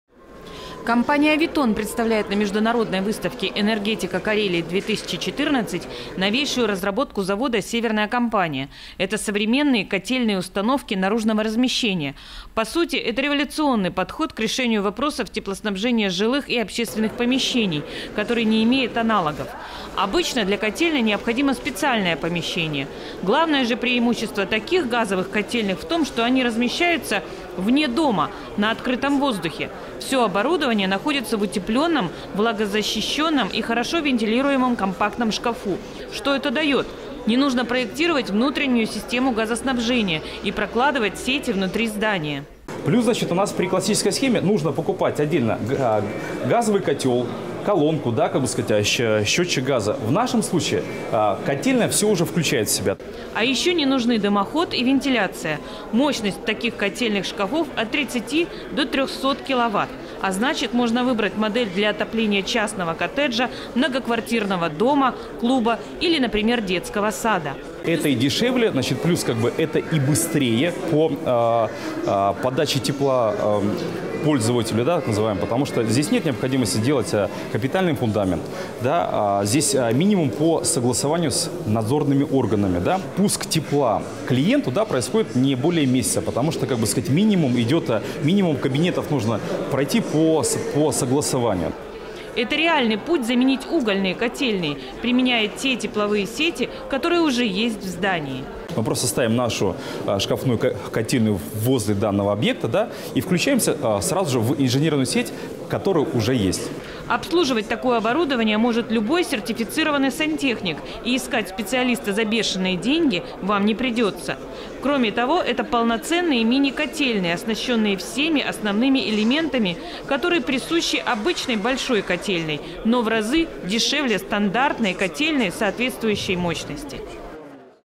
ТГУ-НОРД на выставке Энергетика Карелии
Наш партнер ООО "Авитон" представляет на выставке продукцию производства ООО "Северная Компания" - мини-котельную ТГУ-НОРД.